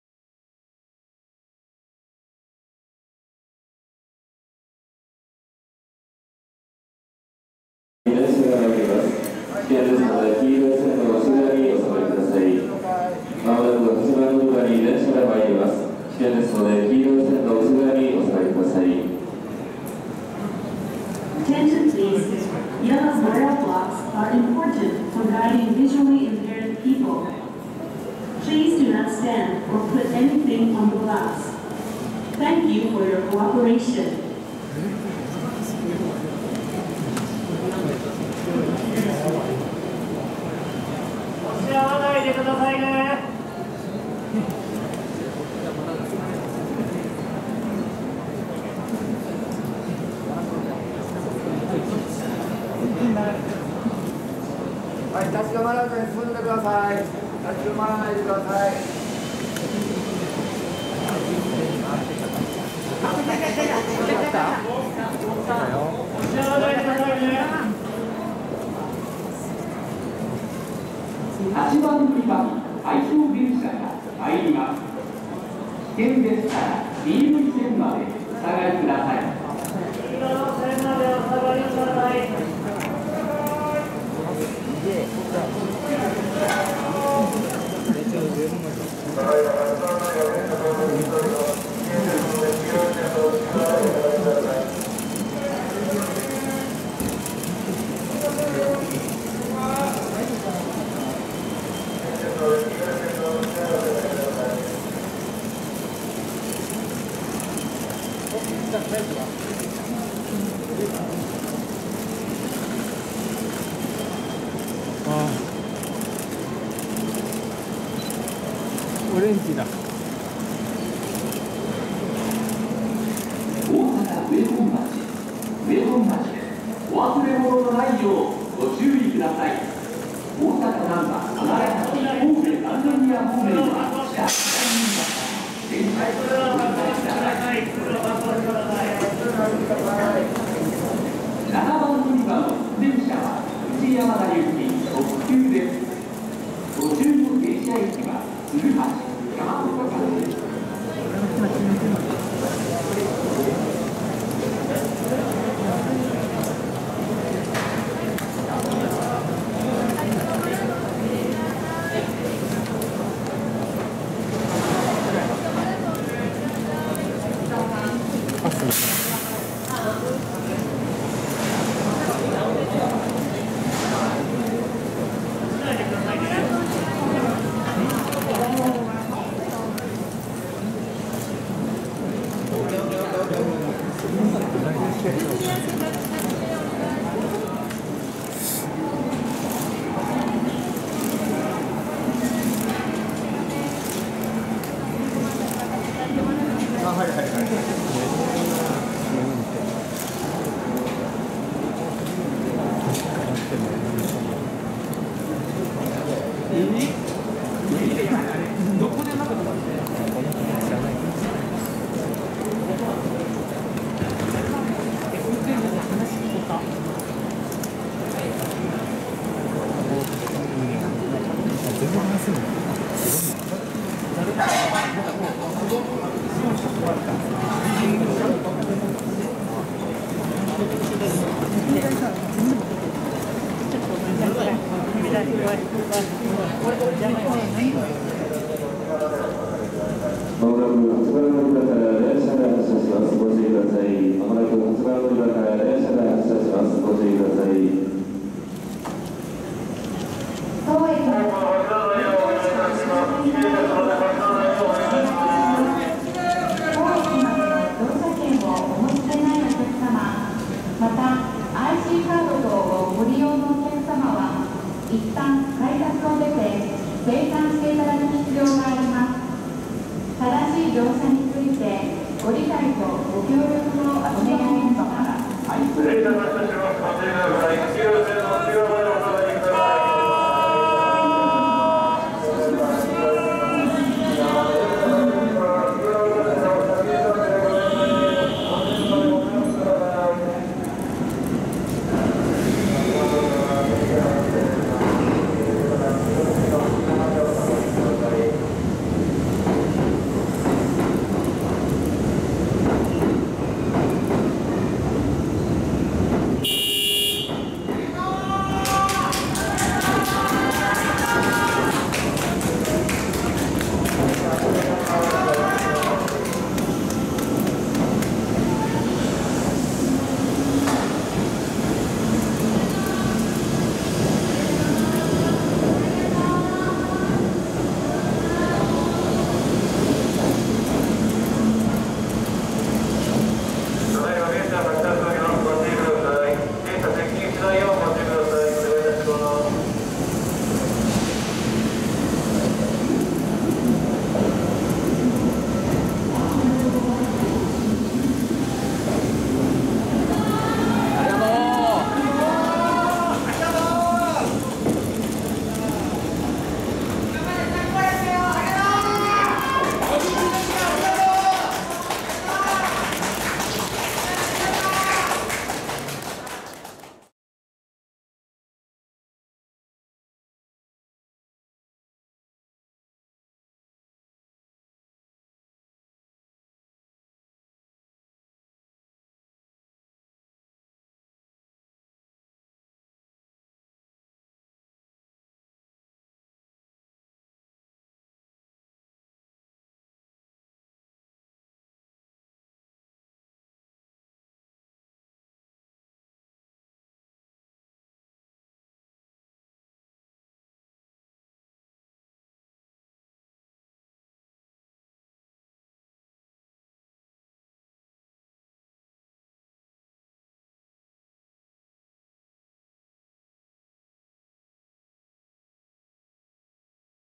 52年間ありがとう！2021年11月20日 近鉄12200系スナックカーラストラン
52年間お疲れ様でした！

1:25…大阪上本町駅に進入
2:12…大阪上本町駅 到着
3:01…ライト切り替えシーン
3:06…幕回しシーン
5:05…大阪上本町駅 発車
6:08…写真集